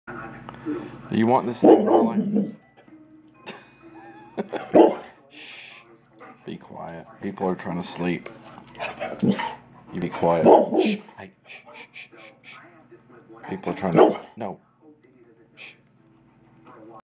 2012-10-19 Youth Season West Texas
Molly ready to hunt in the morning, she saying "strap that collar on!"